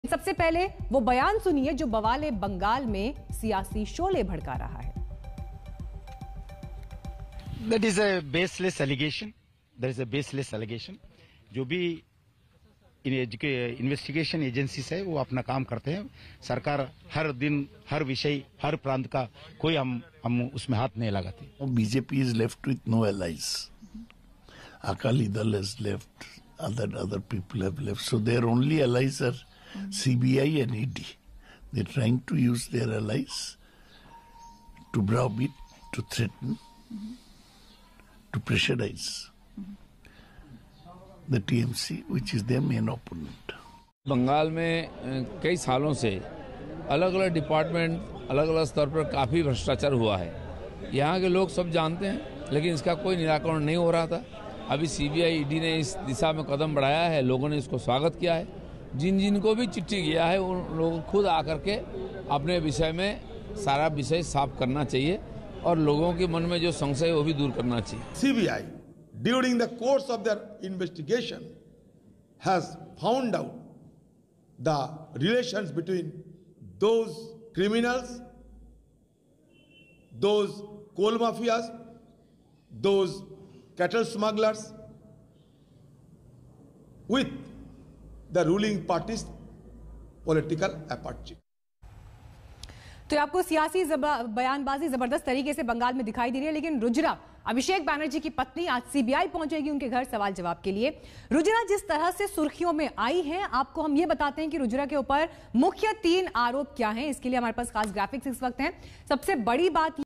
Bengal: Coal Scam में CBI जांच पर सियासत, नेता जमकर लगा रहे एक-दूसरे पर आरोप-प्रत्यारोप
Bengal: कोयला तस्करी केस में आज Mamta Banerjee की बहु यानिकि भतीजे Abhishek Banerjee की पत्नी Rujira Narula से आज CBI पूछताछ करेगी. सोमवार को Rujira से पूछताछ नहीं हो पाई थी. इसको लेकर जमकर सियासत हो रही है.